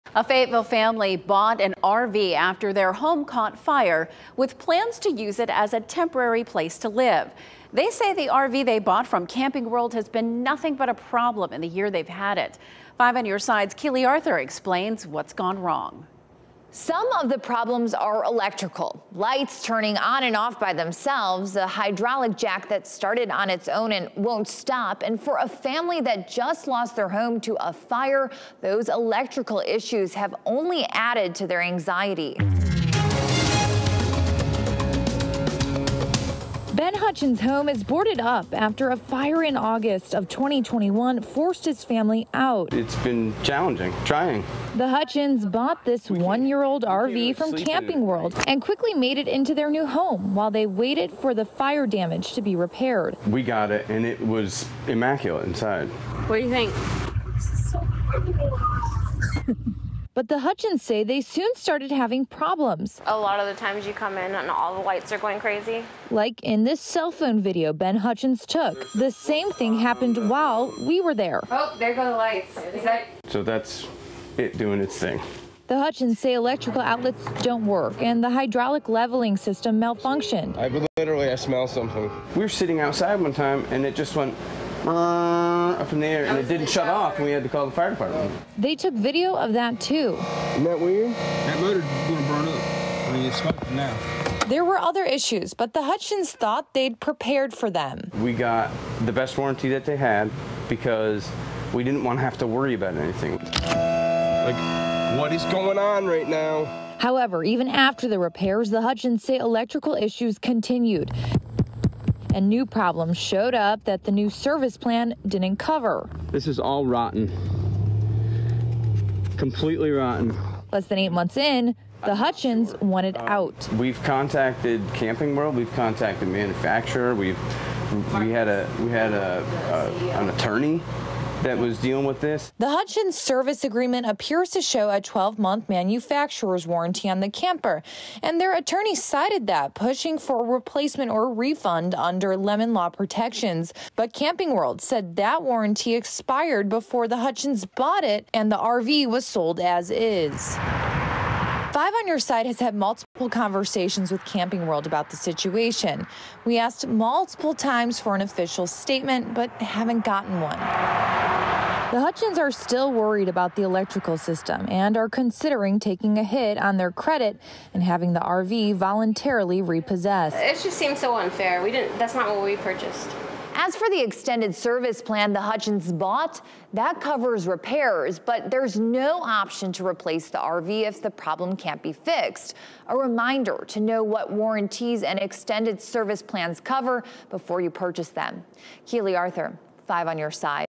Family says RV from Camping World has been nothing but problems
A Fayetteville family says they bought an RV from Camping World to live in after their home caught fire, but they say the RV has been nothing but a problem in the year they've had it.

"It’s been challenging, trying," Ben Hutchins told 5 On Your Side.

In August of 2021, Hutchins’ says the house fire forced his wife and children to find somewhere else to stay.
They bought a one-year-old RV from Camping World and quickly made it into their new home while they waited for the fire damage to be repaired.

"We got it and it was immaculate inside," Hutchins said about the RV.

But the Hutchins say they soon started having problems.

"A lot of the times, you come in and all the lights are going crazy," Heather Hutchins told 5 On Your Side.

Ben Hutchins took cell phone video of the lights flickering and the same thing happened while the 5 On Your Side team was there.

The Hutchins’ say electrical outlets don’t work and at one point, the hydraulic leveling system malfunctioned.

"We were sitting outside one time and it just went ‘brrrr’ up in the air and it didn’t shut off and we had to call the fire department," Ben Hutchins said.